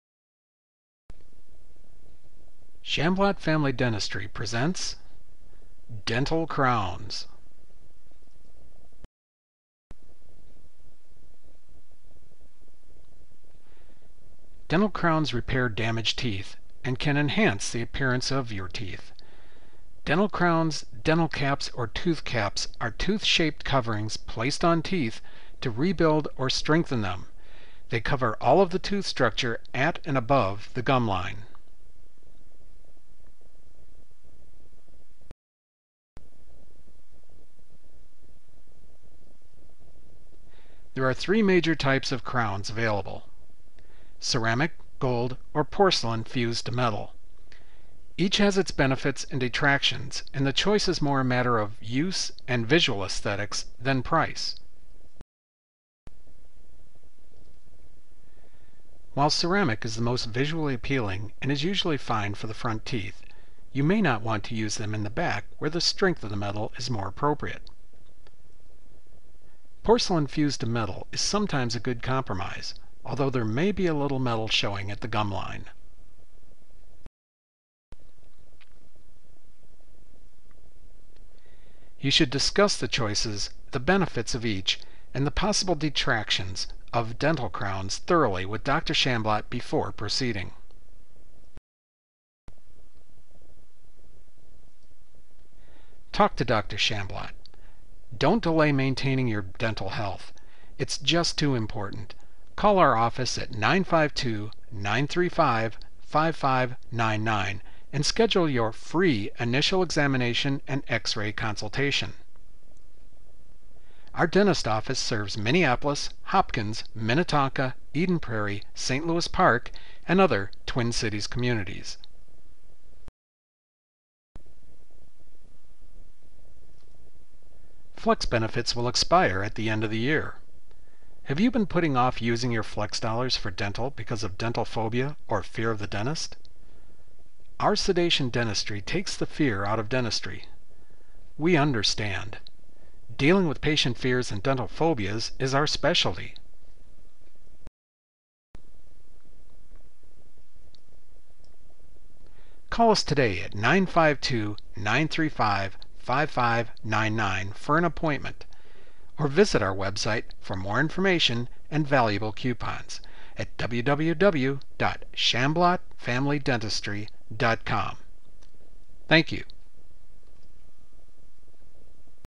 Dental Crowns Can Enhance the Appearance of Your Teeth
Shamblott Family Dentistry - Dental Crowns can enhance the appearance of your teeth. However, it's important to know which dental crowns you should have.